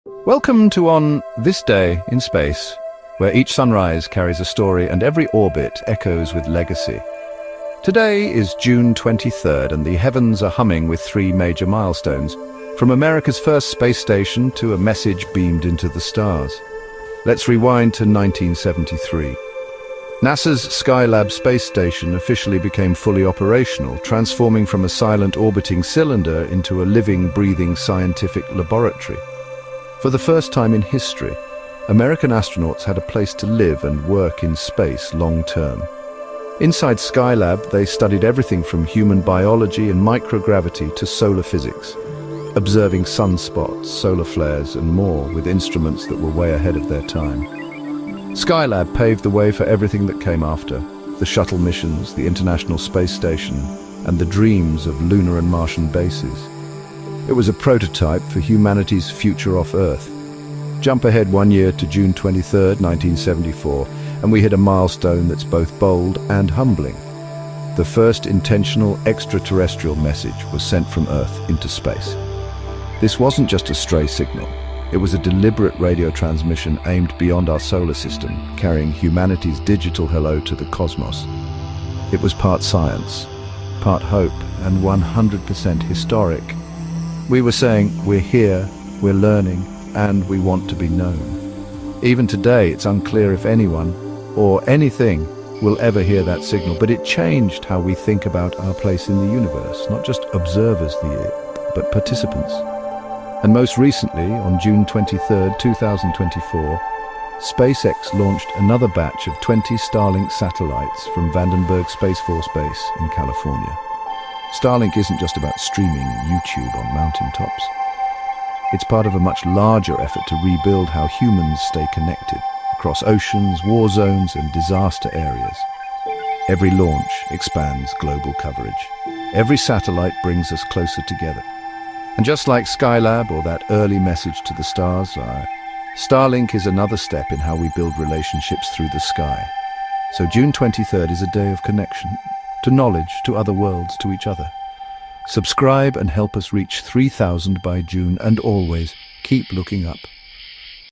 On This Day in Space: 23 June | Skylab, Starlink & Our First Message to the Stars
On June 23:

1973: Skylab became fully operational, beginning America’s first extended stay in space

1974: The first deliberate message to extraterrestrial life was beamed from Earth

2024: SpaceX launched 20 more Starlink satellites from California

This day reminds us that the sky isn’t just a ceiling — it’s a connection.